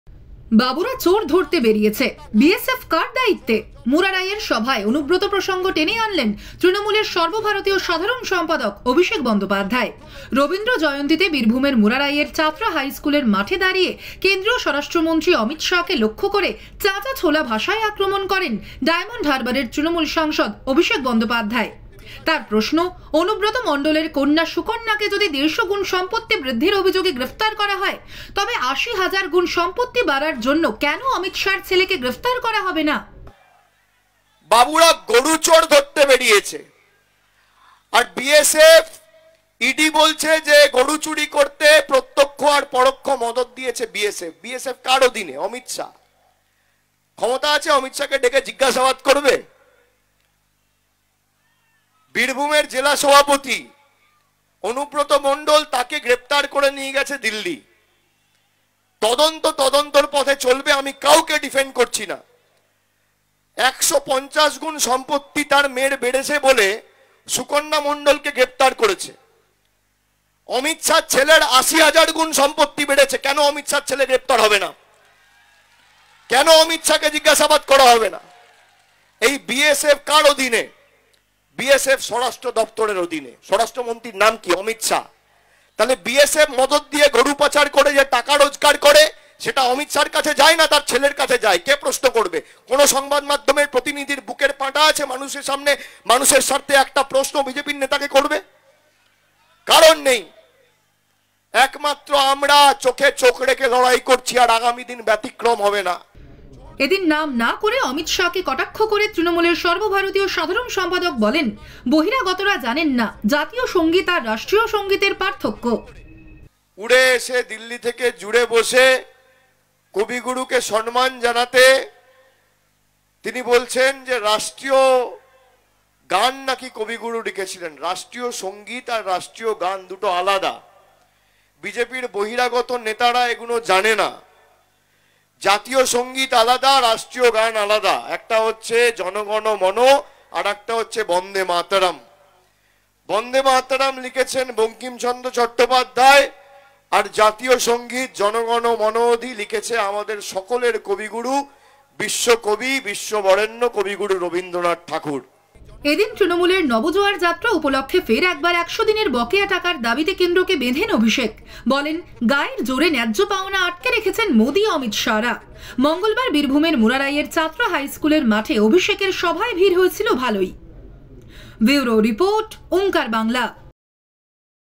"বাবুরা সব চোর ধরতে বেড়িয়েছেন" : অভিষেকের
রবীন্দ্র জয়ন্তীতে বীরভূমের মুরারইয়ের চাতরা হাইস্কুলের মাঠে দাঁড়িয়ে কেন্দ্রীয় স্বরাষ্ট্রমন্ত্রী অমিত শাহকে লক্ষ্য করে চাঁচাছোলা ভাষায় আক্রমণ করলেন অভিষেক বন্দ্যোপাধ্যায়। তৃণমূলের সর্ব ভারতীয় সাধারণ সম্পাদক বলেন বাবুরা সব চোর ধরতে বেড়িয়েছে। বিএসএফ কার দায়িত্বে ? অনুব্রত মণ্ডলের কন্যাকে যদি ১৫০ গুন সম্পত্তি  বৃদ্ধির অভিযোগে গ্রেফতার করা হয় তবে ৮০ হাজার গুন সম্পত্তি বাড়ার জন্য কেন অমিত শাহের ছেলেকে গ্রেফতার করা হবে না।